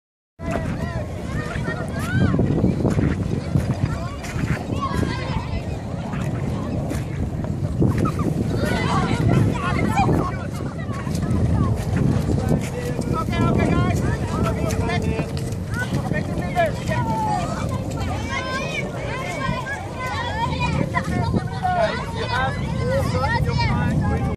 Al Reem School Kayaks
Al Reem School students are kayaking and surveying the mangroves of Yas Island in Abu Dhabi